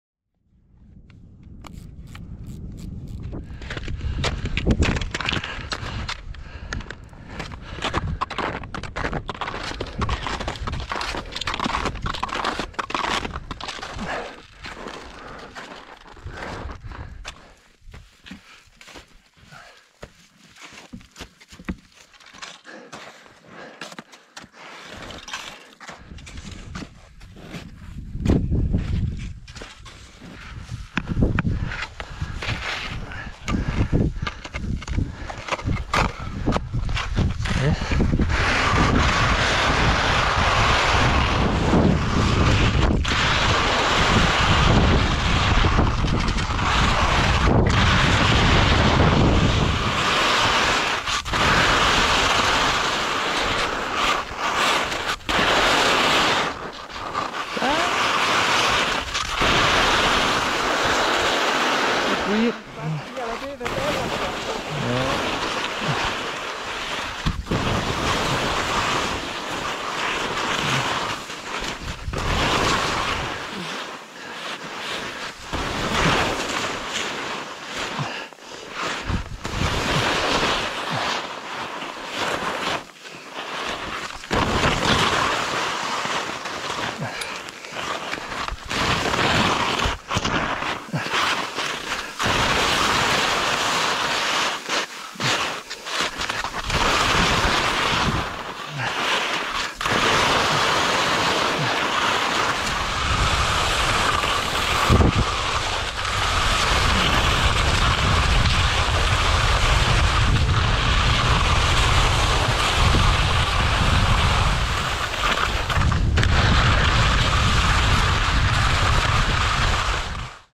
Brenva Spur "one shot" - Denis Trento
Every end is a new beginning. After 50 days of confinement, our athlete Denis Trento was finally able to fulfil one of his dreams: climbing and skiing the Brenva Spur in one push. Don't miss the article on grivel.com: